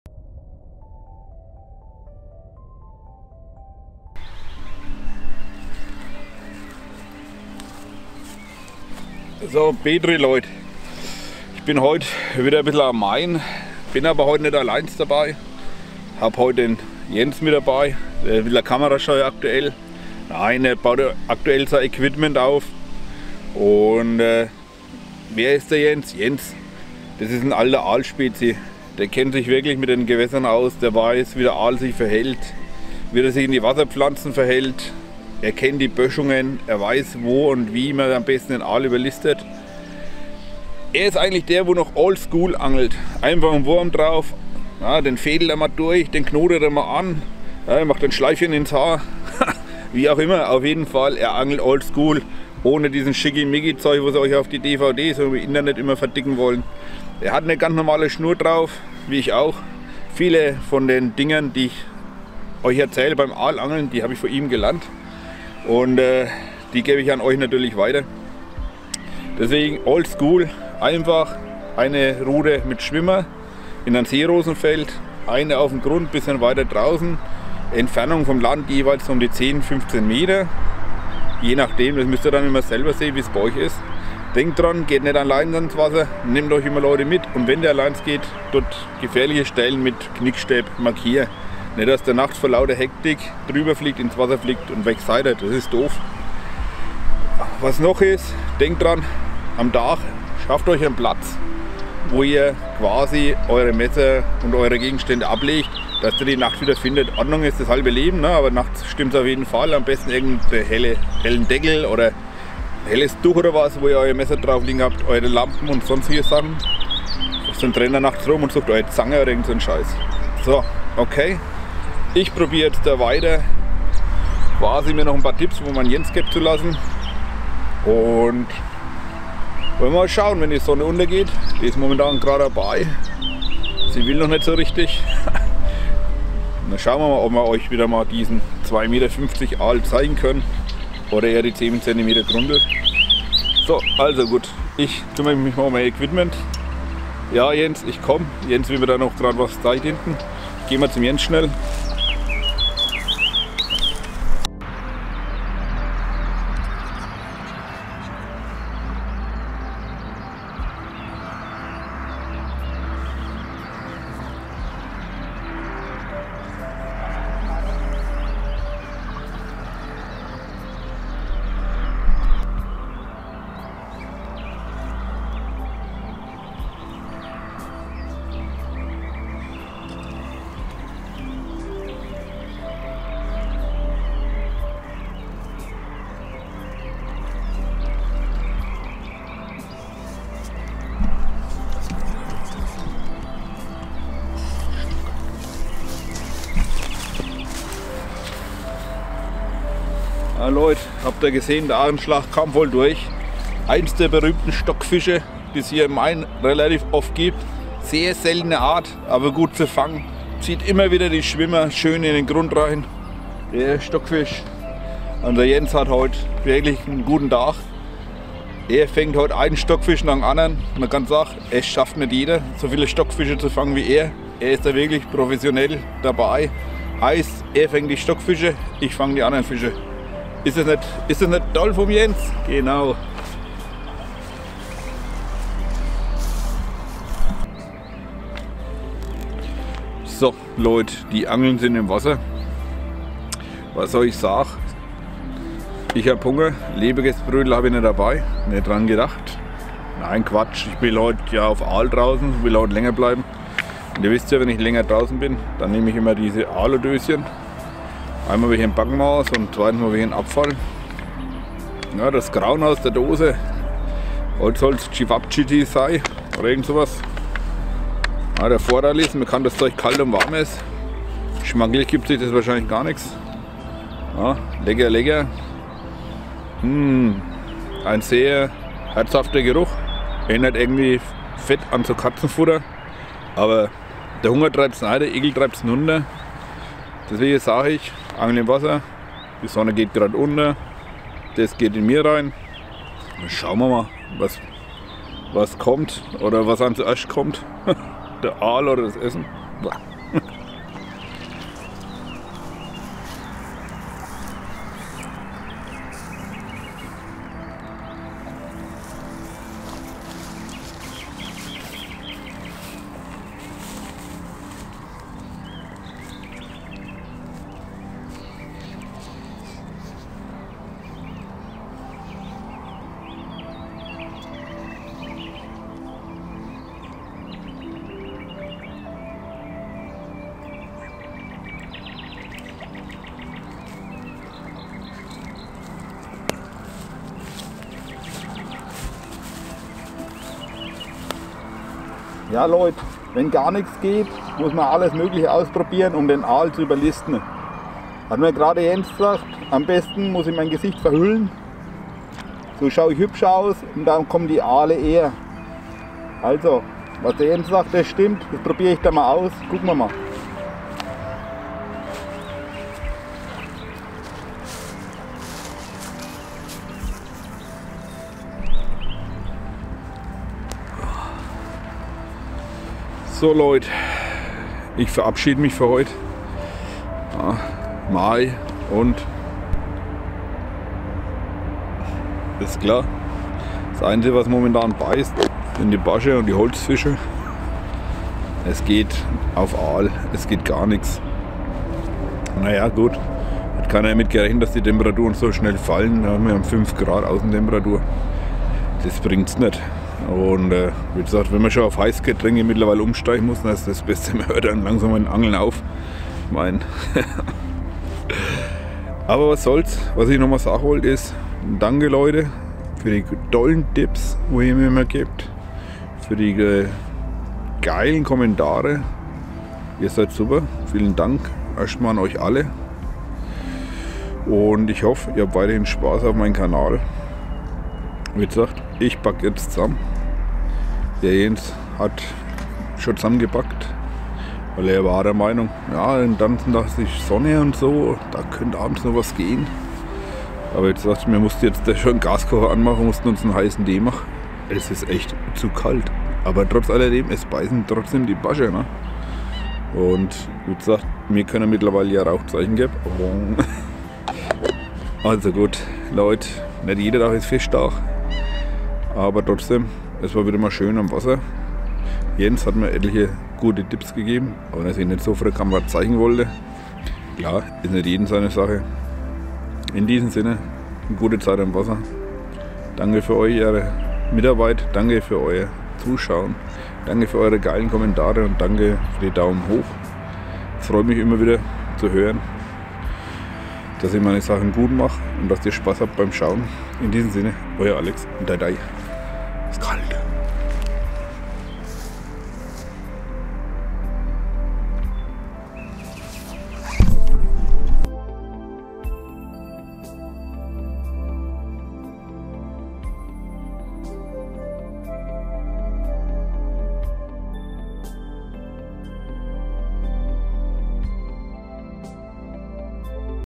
Der Holzfische am Angelhaken
Hier zeigt uns  Jens wie man ihn fängt den " Holzfisch " es ist ein Art die doch jeder kennt .